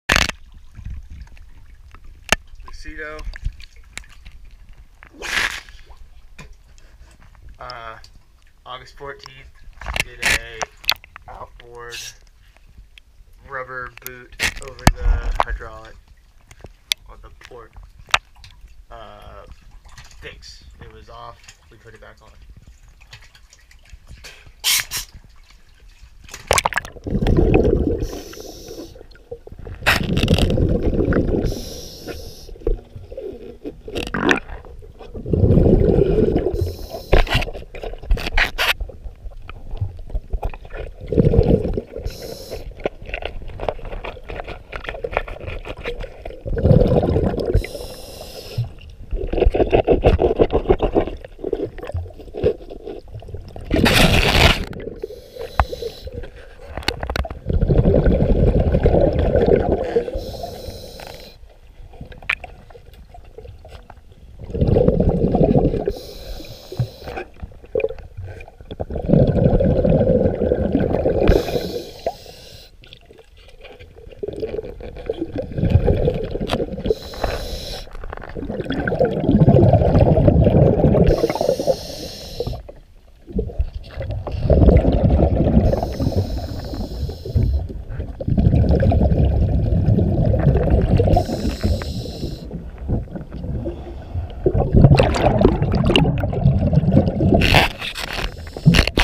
8 14 18 Lucido Cleaned and rubber boot fixedMP4
We were able to re-seat the port outdrive trim ram boot and get her all cleaned up